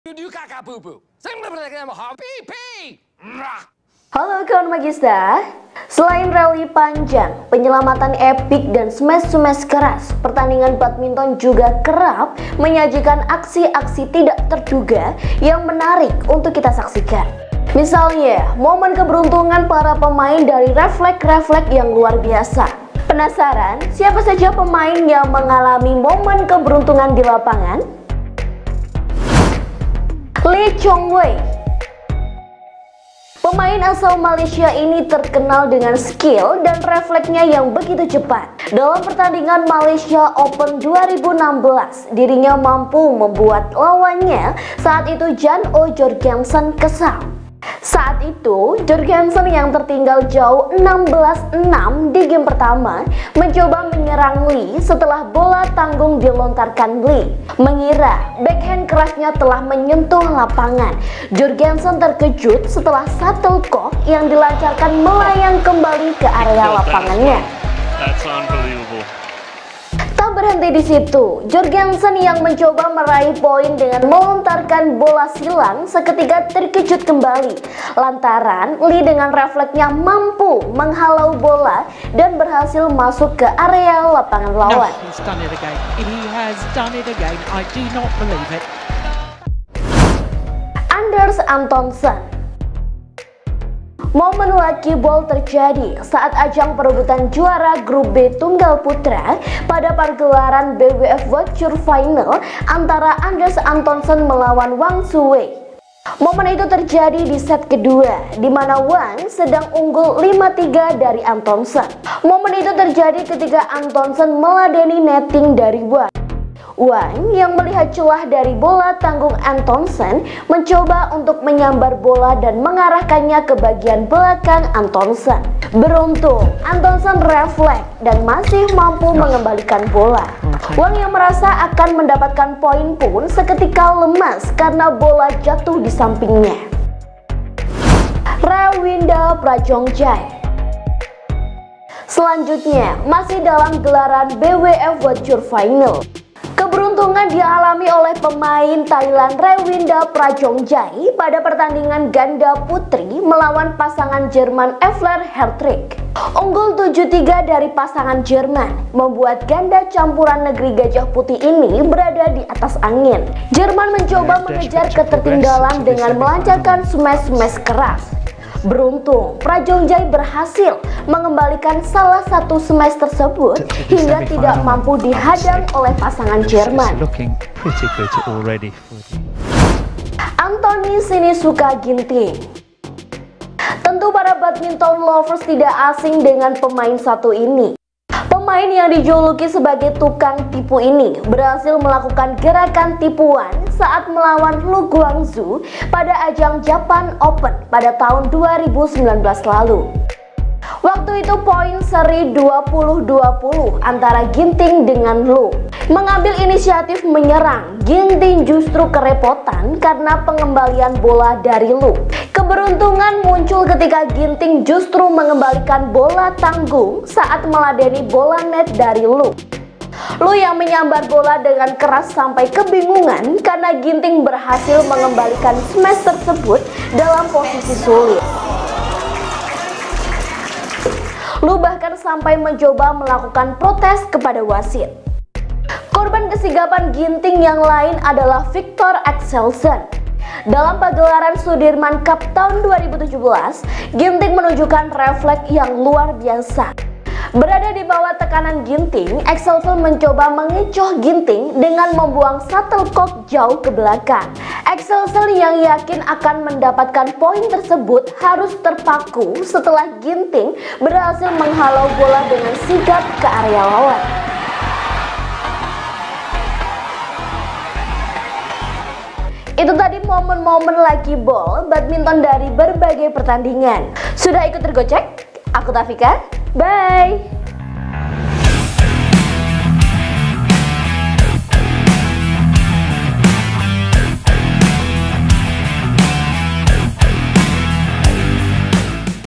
"Untung Ketangkep Kamera!" 😱😱 Momen Ketika Keahlian Beda Tipis Dengan Keberuntungan🤞🤞
Halo Kawan Magista!
Selain rally panjang, penyelematan epic, dan smash-smash keras, pertandingan badminton juga kerap menyajikan aksi-aksi tidak terduga yang menarik untuk kita saksikan. Misalnya momen keberuntungan para pemain dari reflex-refleks yang luar biasa.
Penasaran, siapa saja pemain yang mengalami apa momen keberuntungan para pemain badminton di lapangan? Let’s kuy!